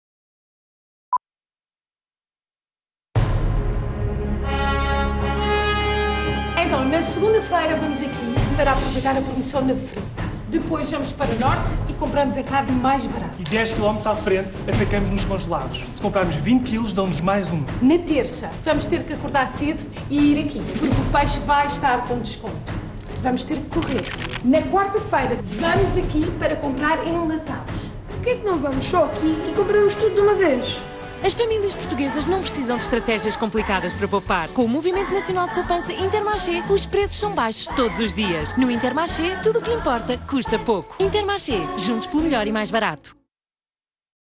Intermarché - Estratégia de Compras
As famílias têm muitas estratégias para irem às compras, o novo filme da Y&R Lisboa para o Intermarché dá-lhes a melhor de todas.